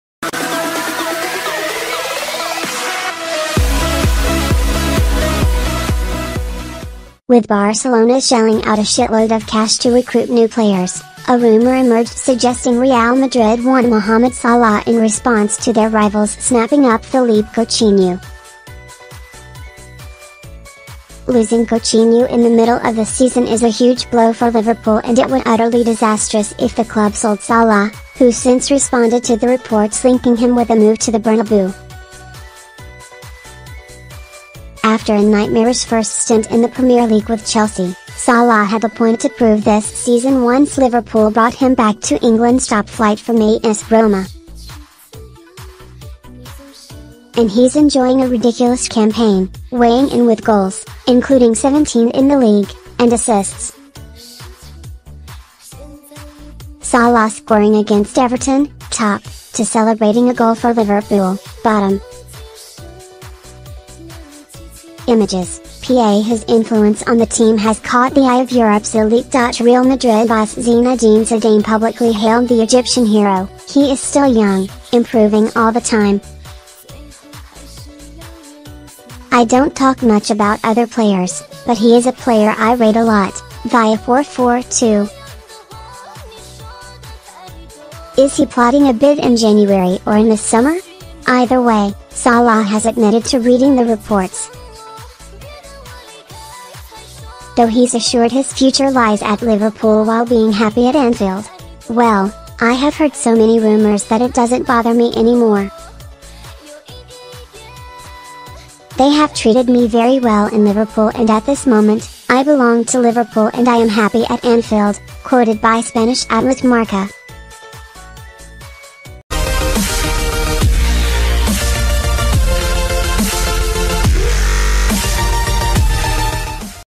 Mohamed Salah Reacts To Insane Real Madrid Transfer Rumour
With Barcelona shelling out a shitload of cash to recruit new players, a rumour emerged suggesting Real Madrid want Mohamed Salah in response to their rivals snapping up Philippe Coutinho. Losing Coutinho in the middle of the season is a huge blow for Liverpool and it would utterly disastrous if the